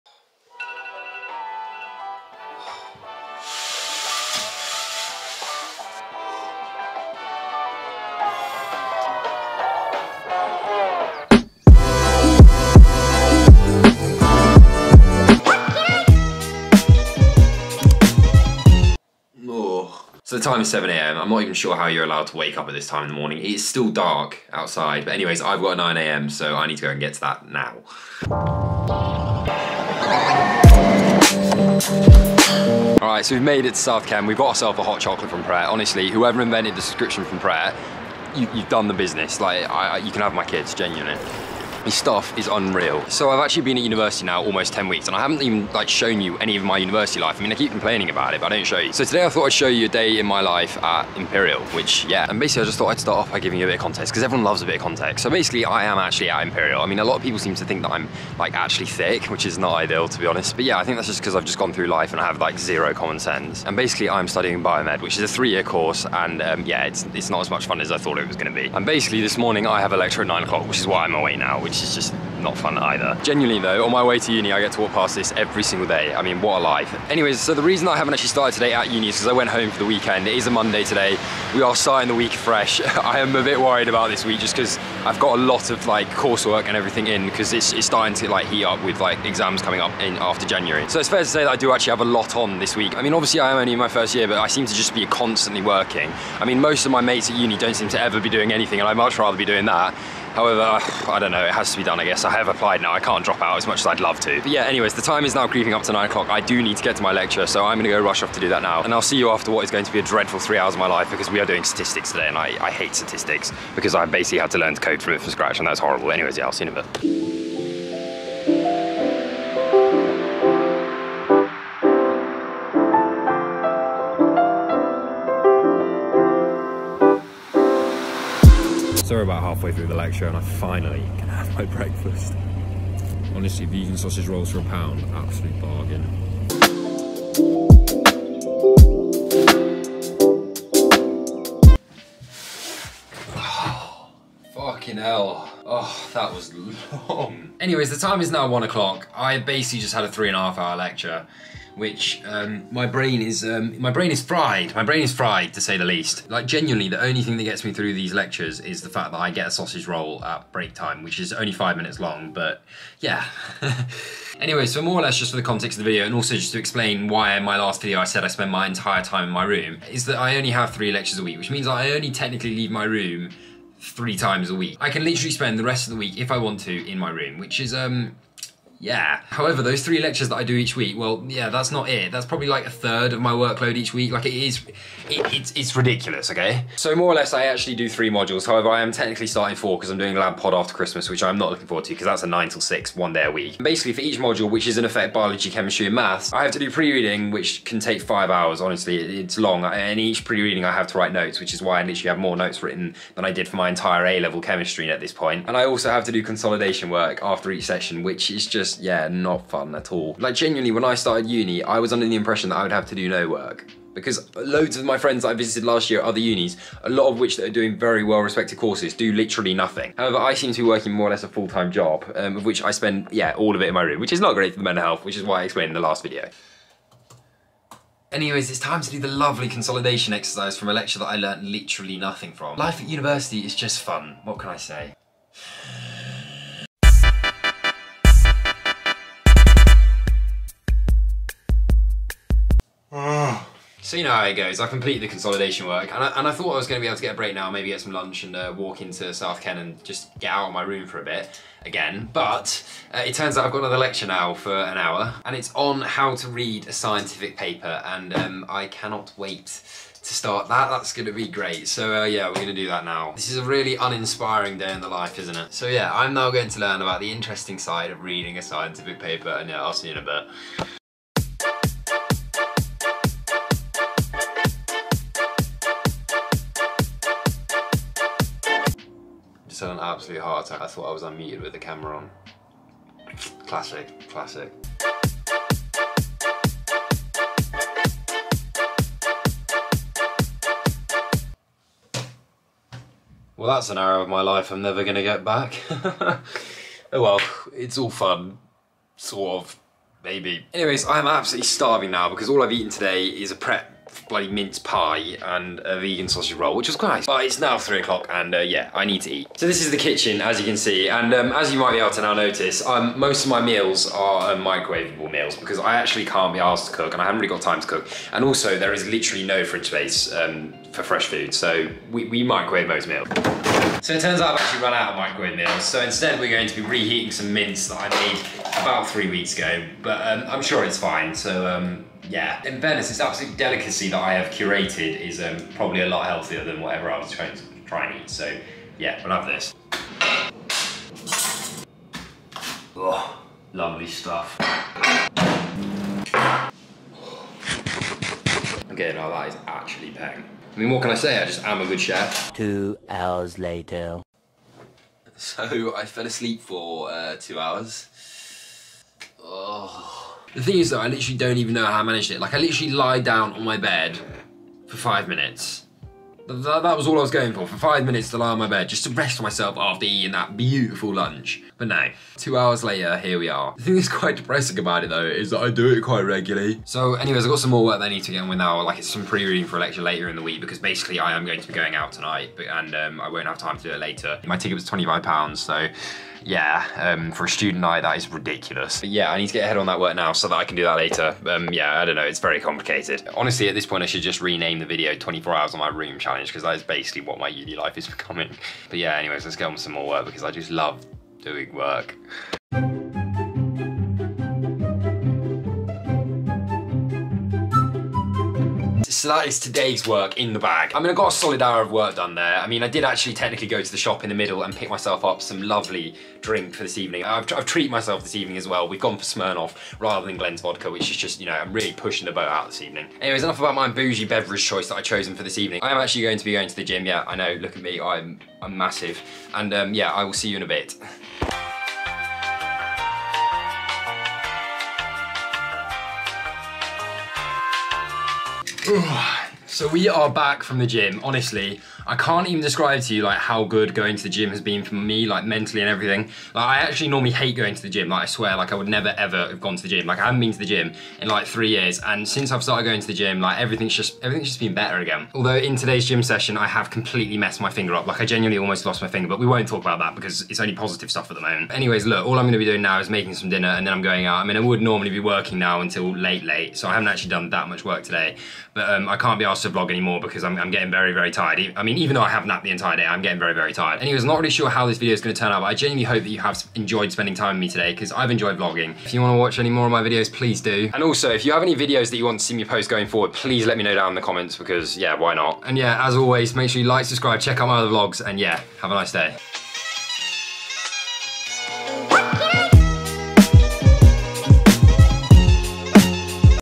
DAY IN MY LIFE at IMPERIAL COLLEGE LONDON : *what's it like?*
So I started uni in London in September without really knowing what I was getting myself into. I am studying Biomed ant imperial college London which is one of the unis in London and I thought I would share with you what a day in my life looks like.
In this video I discuss my mental health and how I've been dealing with it but also how to stay on top of your uni work but also how to get into imperial college London.

In this video I also show what uni food im cooking and how to stay healthy at university

Thanks
James E